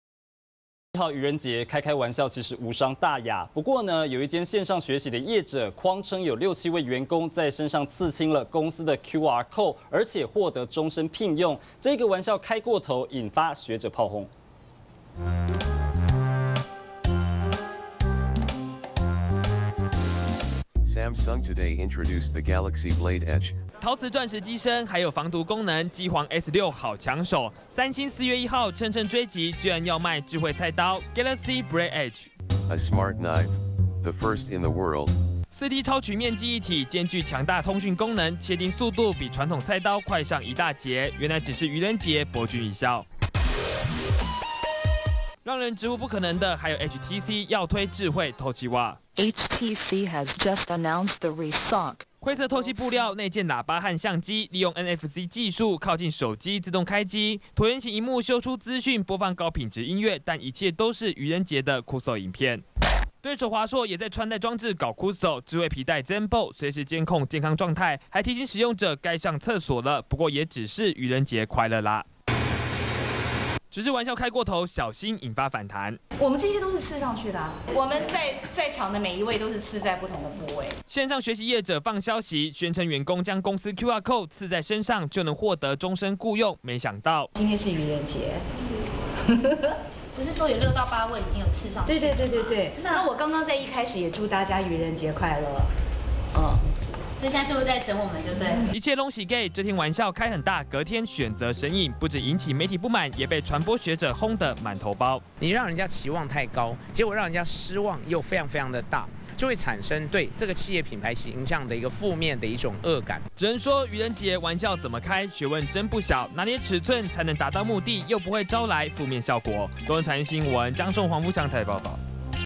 愚人節!三星"智慧菜刀" hTC"智慧襪"搞kuso
昨天4月1號是愚人節，手機大廠紛紛推出，超吸睛產品，像是三星的"智慧菜刀"，和宏達電的"智慧襪"，其實都只是搞KUSO，想搏君一笑，不過也有線上學習業者，玩笑開過頭，誆稱有6-7位員工，在身上刺青，公司QRCODE，獲得終身聘用，結果一切攏係假，引發學者砲轟！